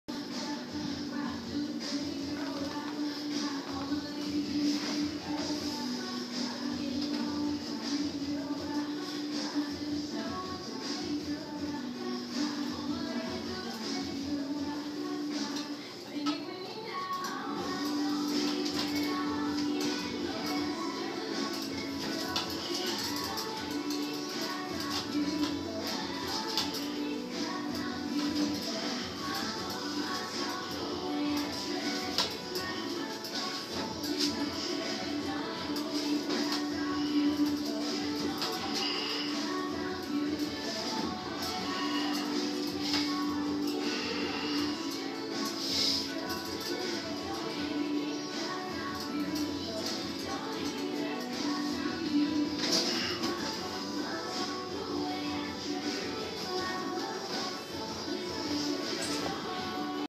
Trying new workouts
Trying new workouts with different  equipment and in different positions